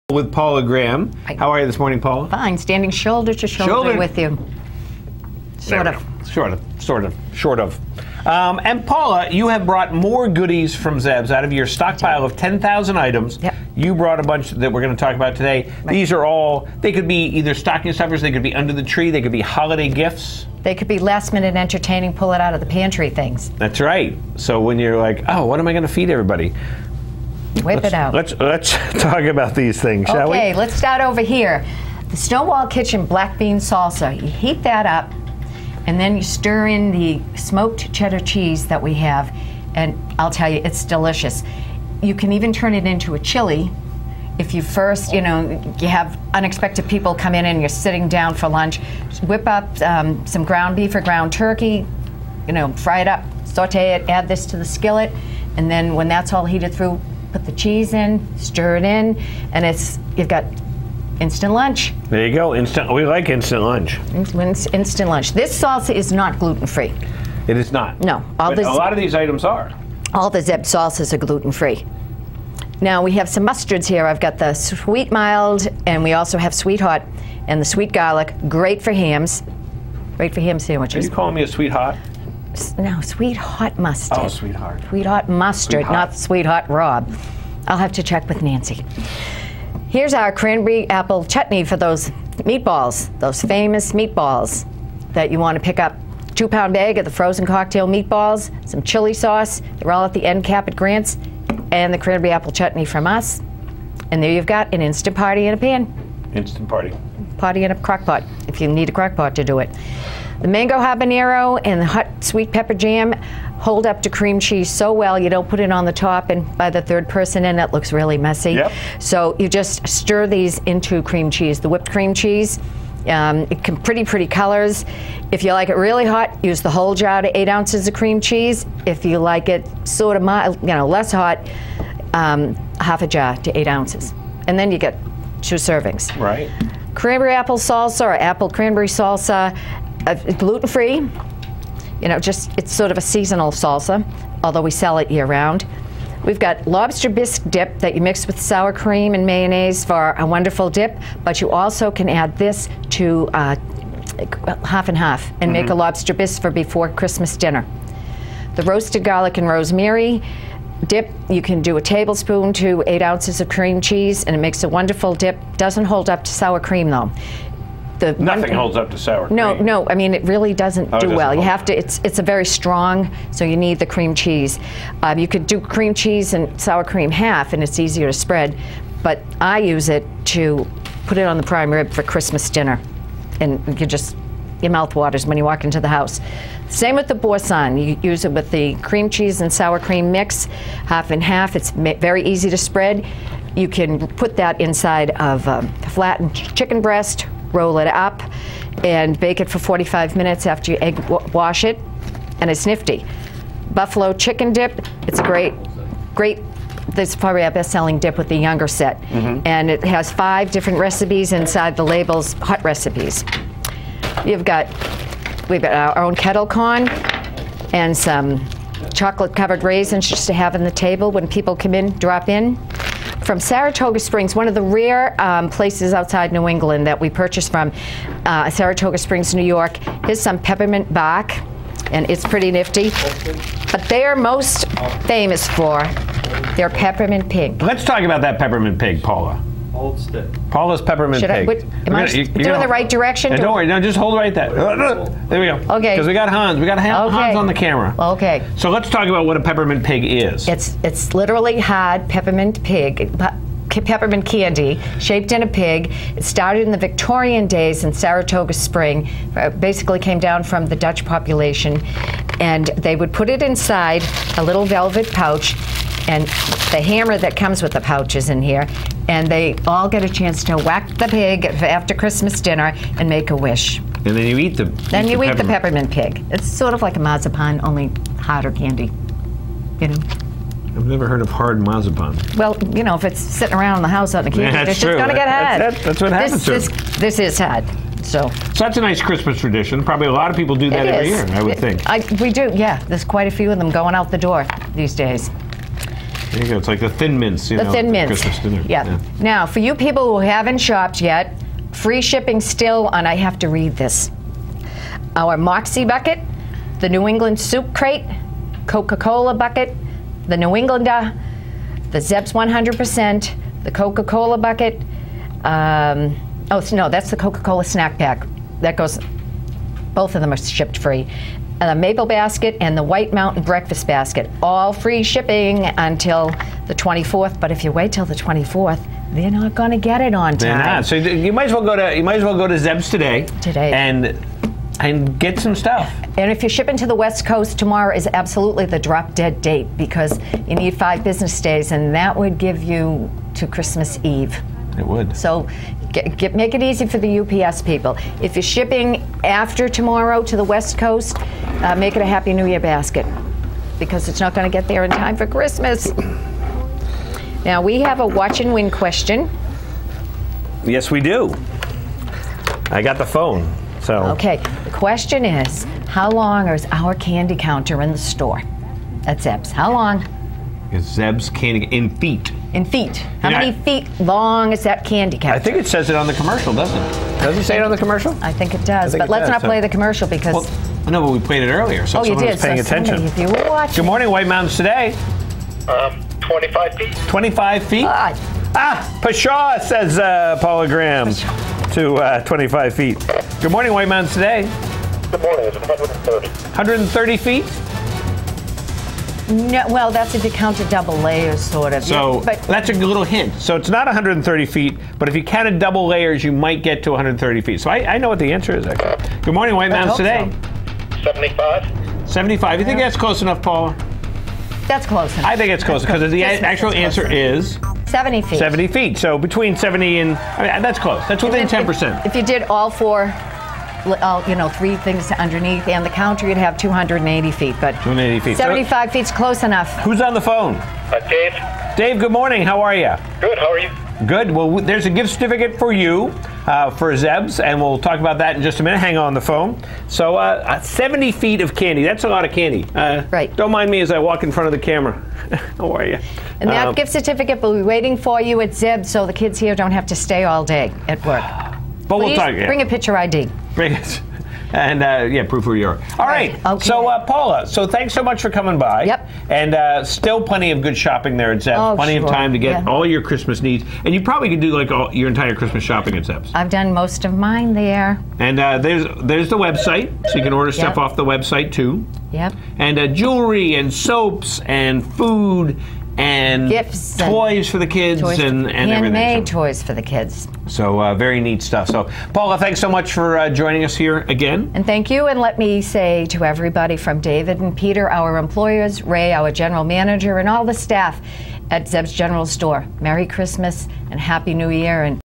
Zeb's General Store
Paula Graham talks about another collection of items that can be found in the store and online at  zebs.com.